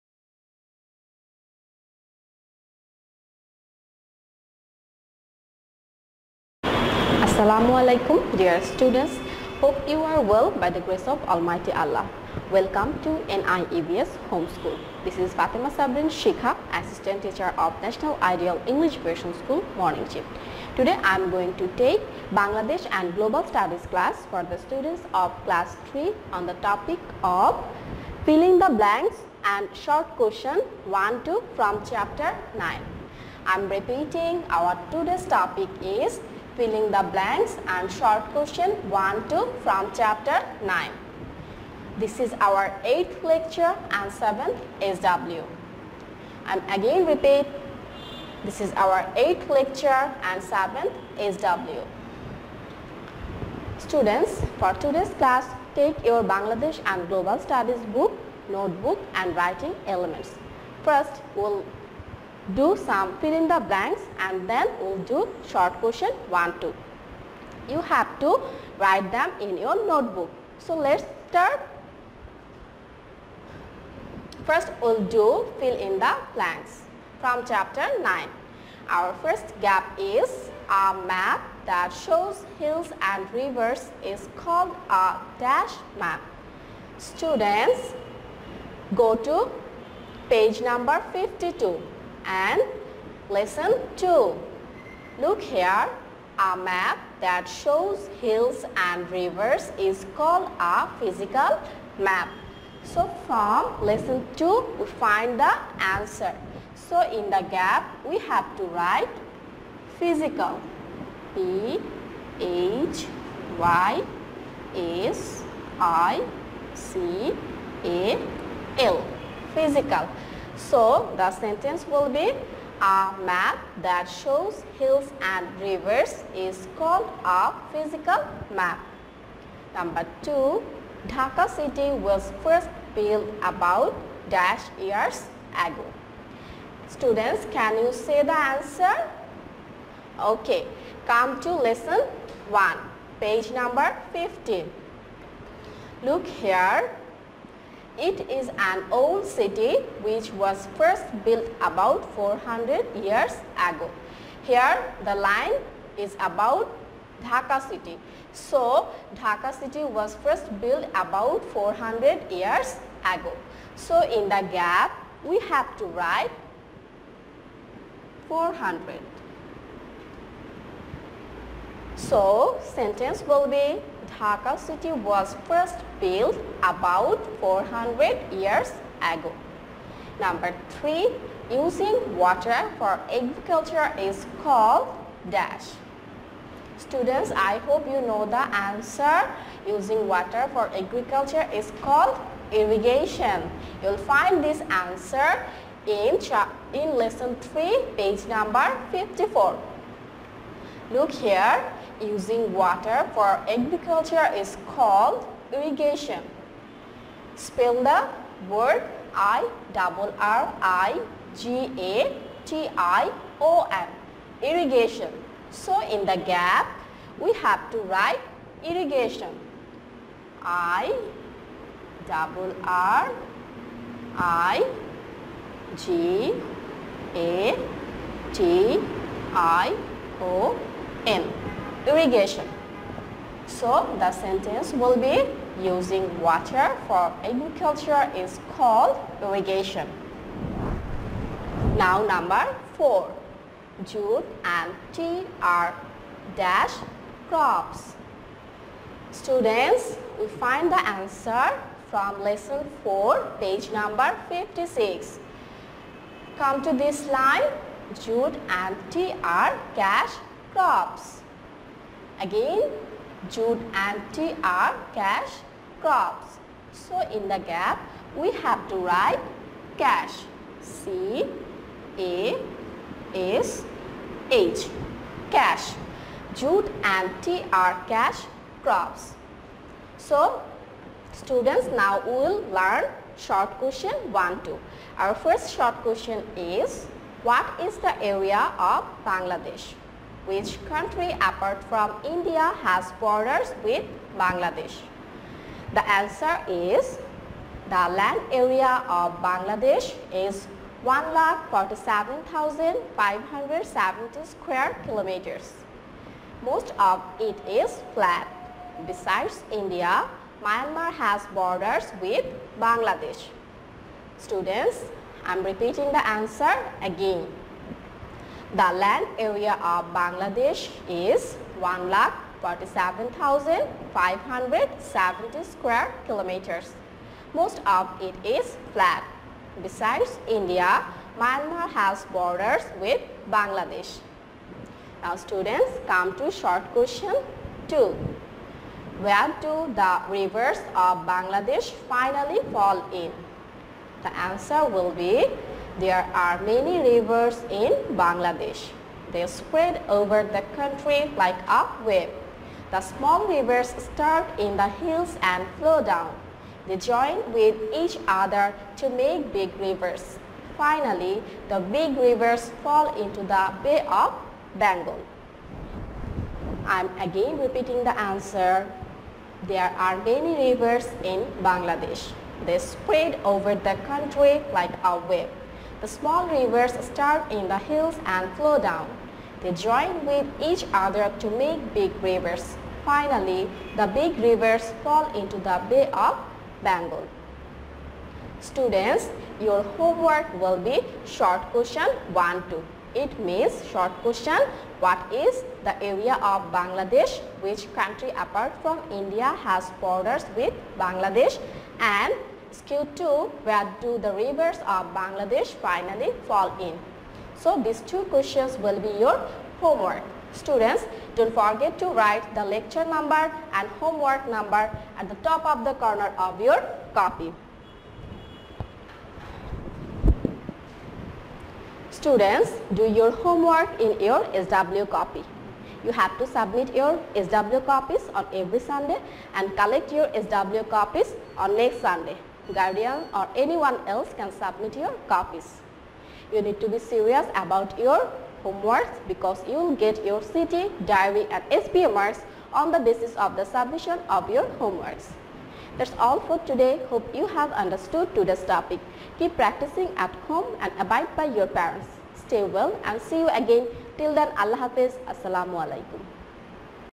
Class: Three, Sub: Bangladesh and Global Studies (Lee- 8, H.W.- 7), Topic: Chapter-9, F/B+S/Q- 1, 2
Annual Term
Class: Three
Subject: Bangladesh and Global Studies (Lecture- 8, H.W.- 7)
Topic: Chapter-9, F/B+S/Q- 1, 2
Teacher: Fatema Sabrin Shikha